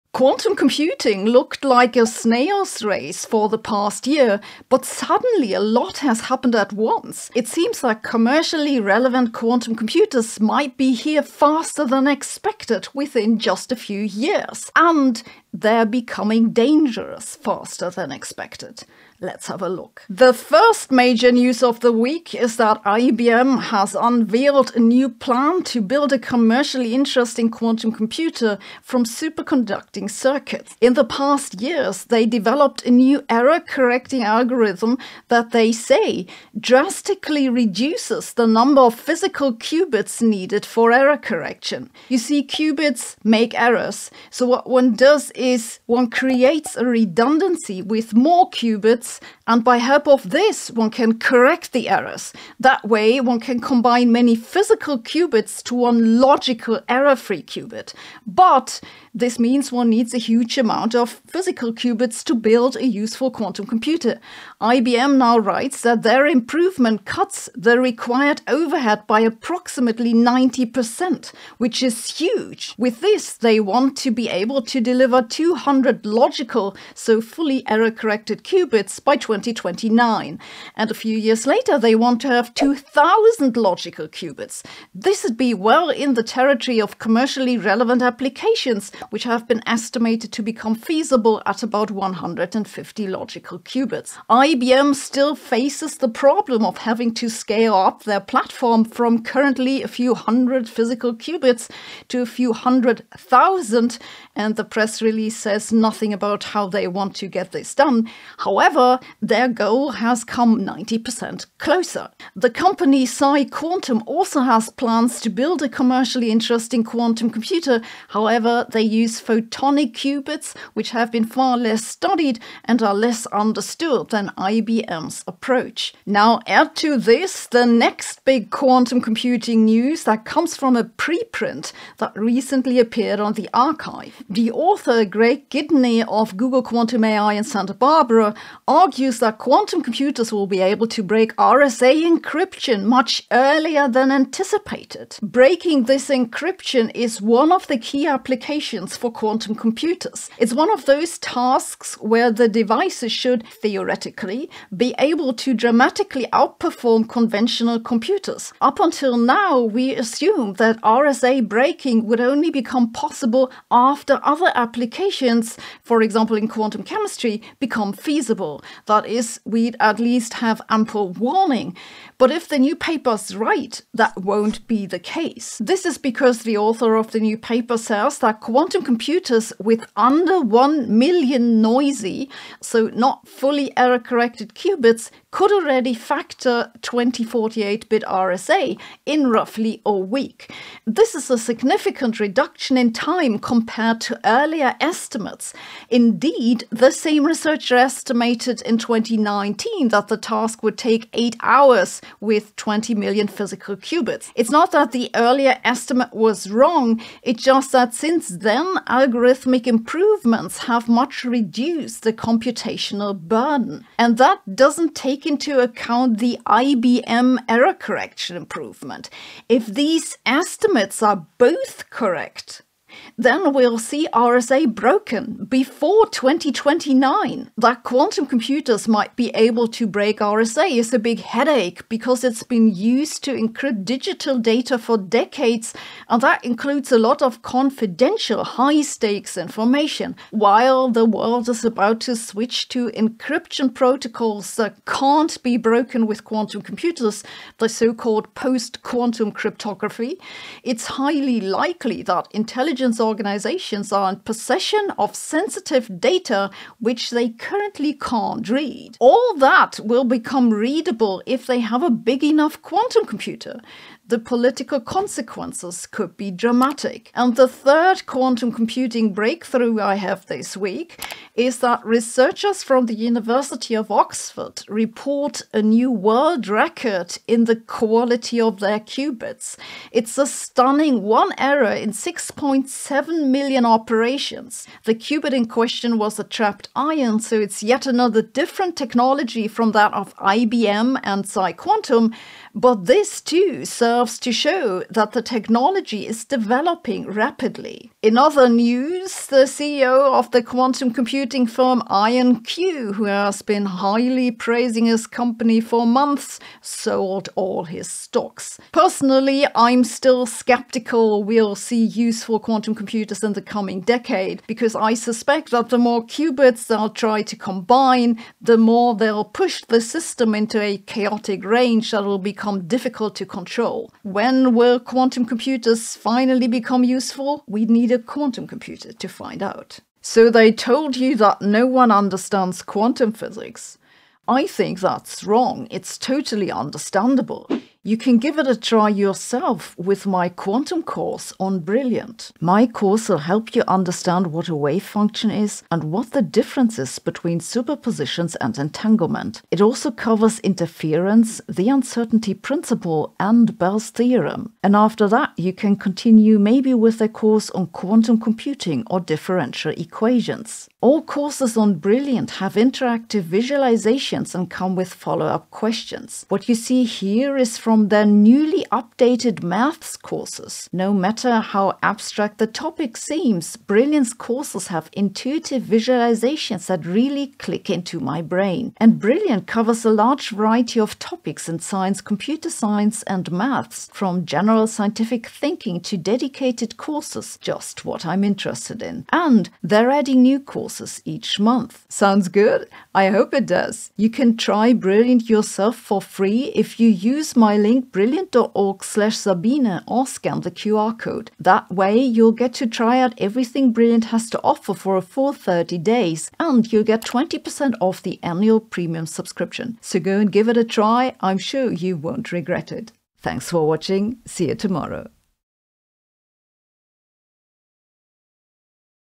Surprise Progress in Quantum Computing
Quantum computing companies always claim that the next big breakthrough is around the corner. Usually you can safely ignore those claims, but a recent series of quantum computing developments might mean that commercially useful quantum computers could be just a few years off. And it looks like they are becoming dangerous faster than we thought. Let’s take a look.